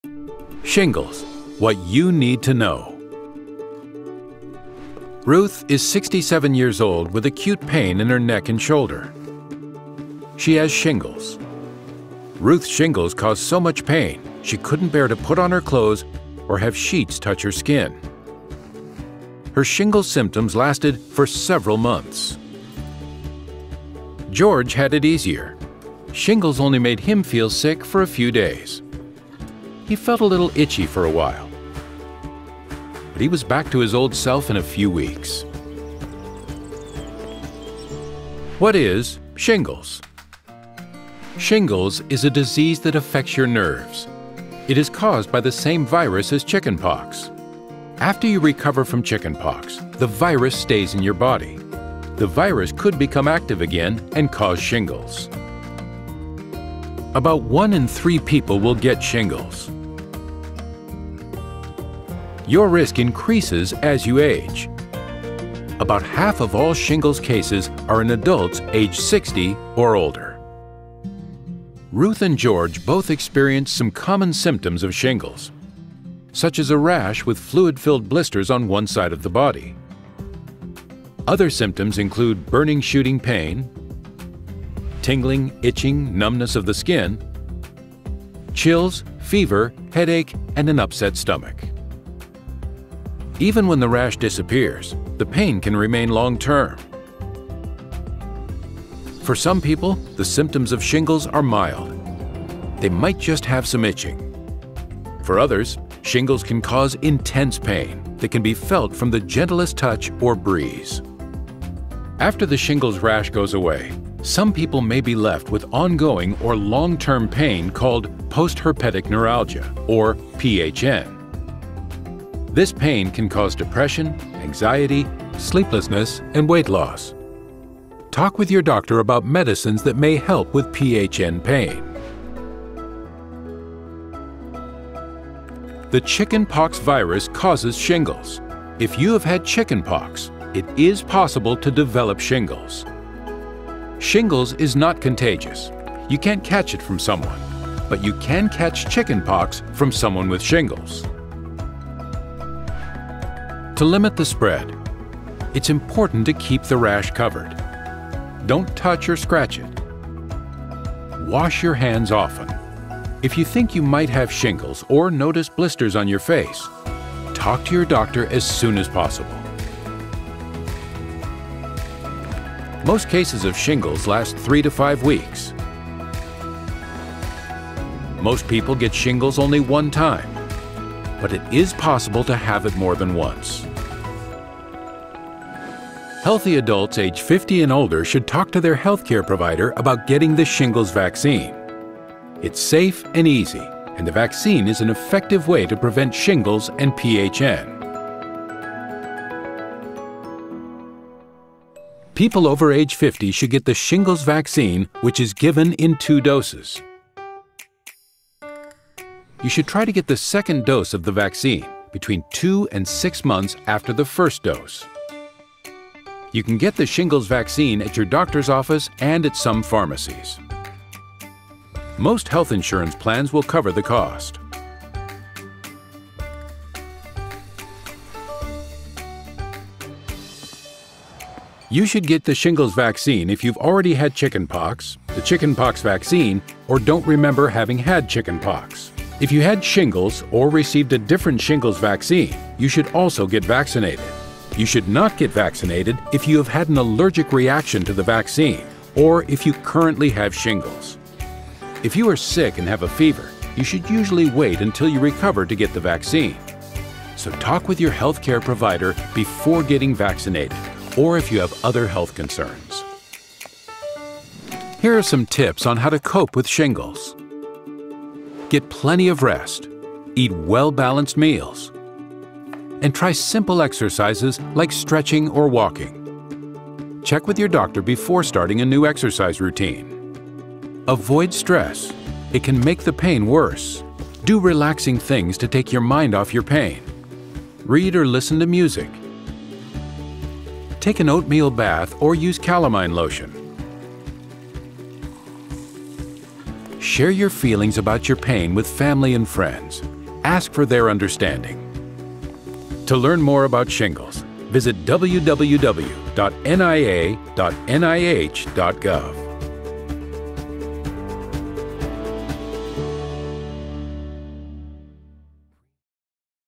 Shingles: What you need to know about causes, symptoms, and prevention.
Learn about the causes, symptoms, and prevention of shingles, including the shingles vaccine. View this video in Spanish